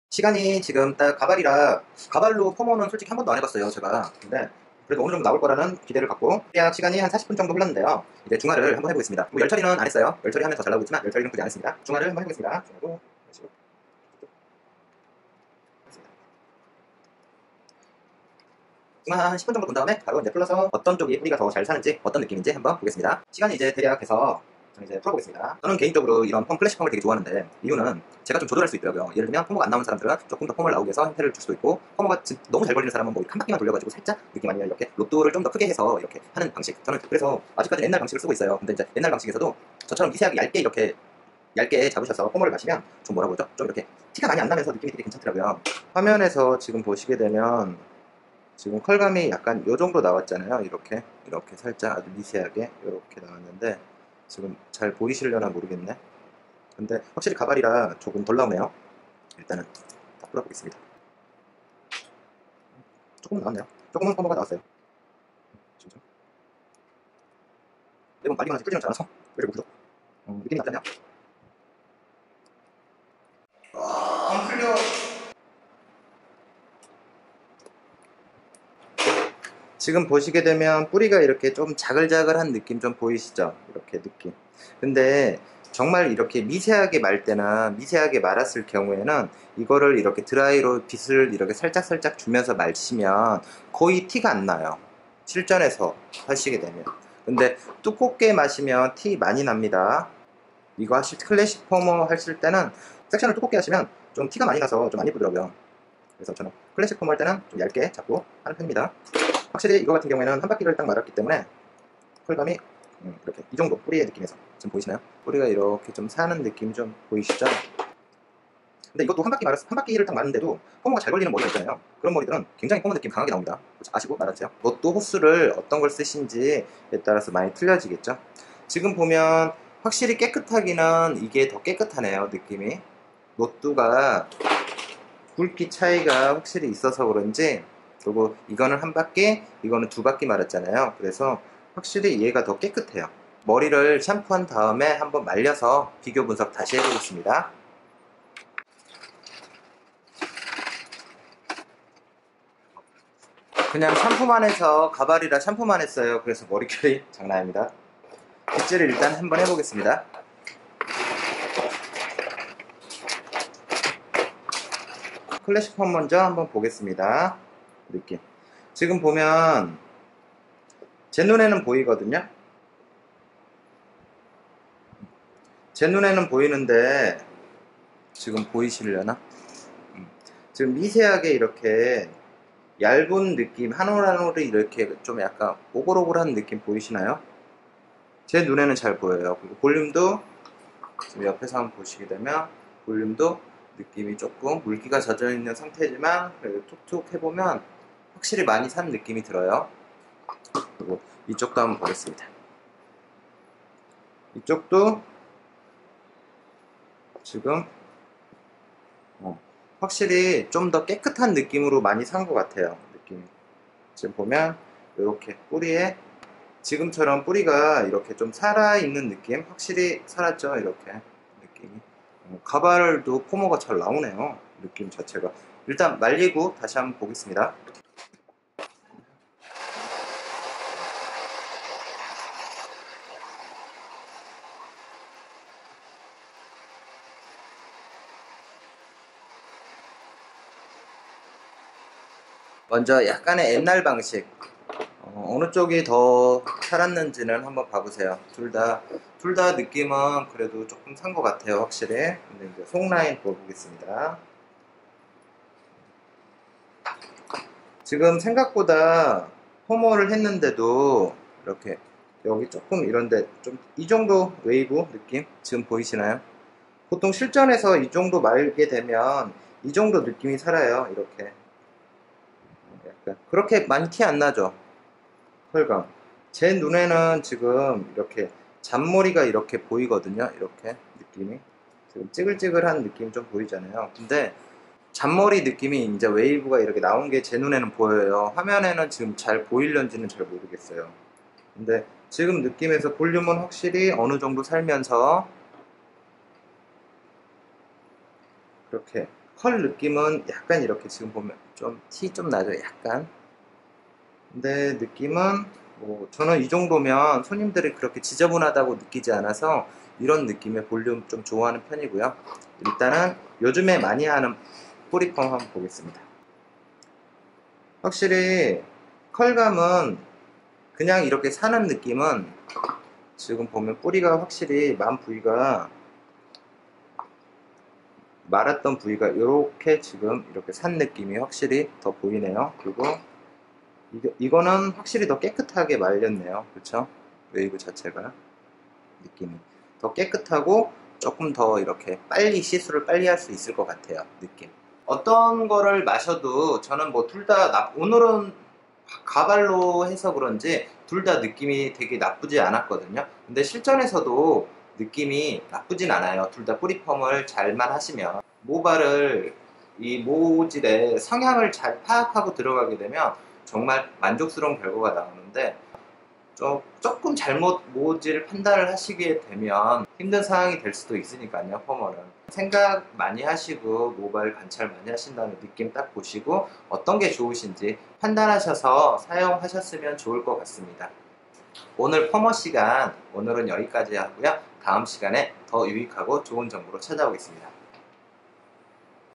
2편 뿌리볼륨펌 비교 분석 뿌리펌 하는 방법 Dby susung 수성원장 ❤️
❤️안녕하세요 헤어디자이너 수성입니다👋^^오늘은 뿌리펌 종류에 대해서 한번 알아보겠습니다 제가 현장에서 쓰는 느낌으로 연출을 해 보았습니다

주소.      청담동 78- 5 번지 2층 디바이수성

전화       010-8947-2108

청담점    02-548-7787